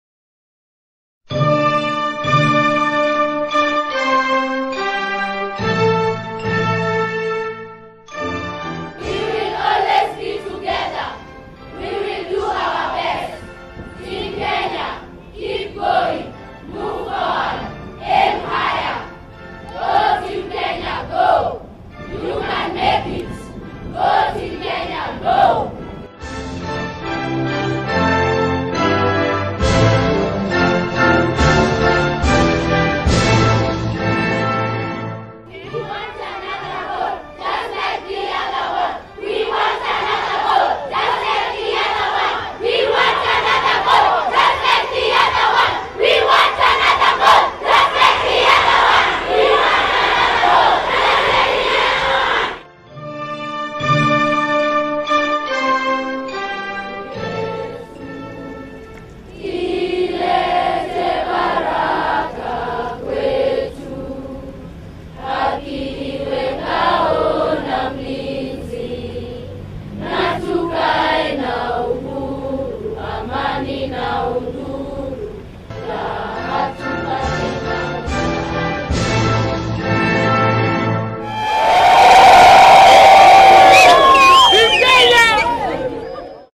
Team Kenya Cheer
Kenyans rally and cheer in continuous support for Team Kenya at London Olympics.